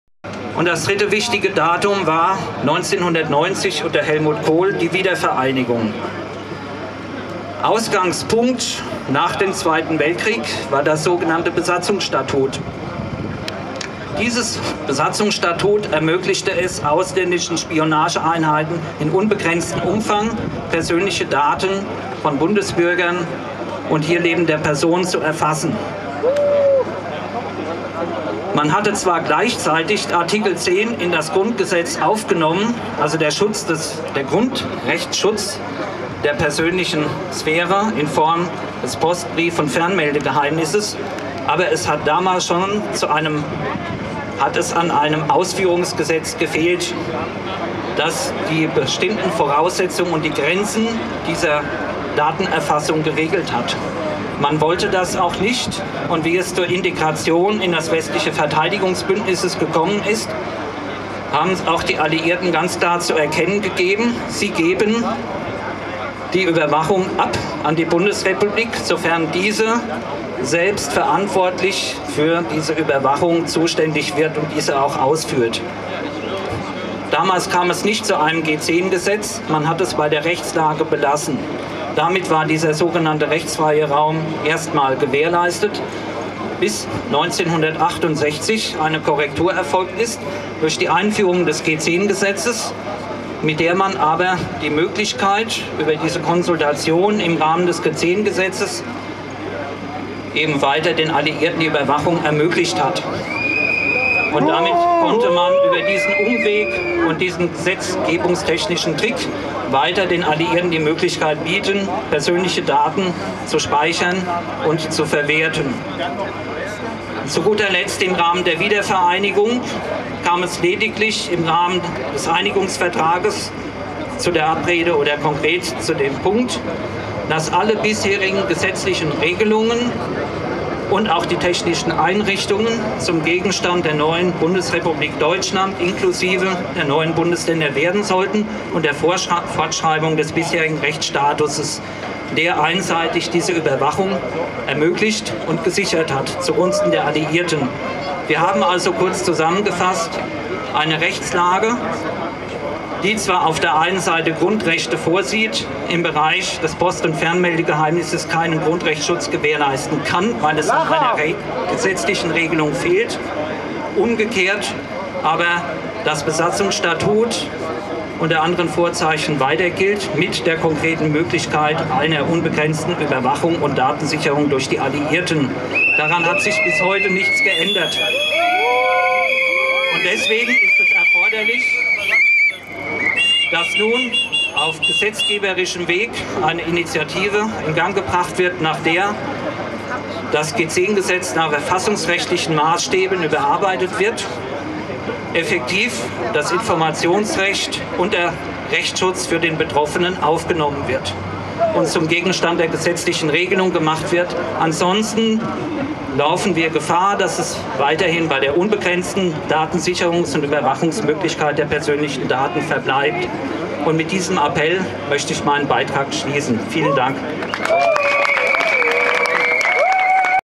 Manfred Hanesch (VDJ) am Dagger, 29.03.14, Teil 2
Herr Hanesch spricht bei der Auftaktkundgebung zum Protestmarsch am Dagger-Complex in Griesheim bei Darmstadt.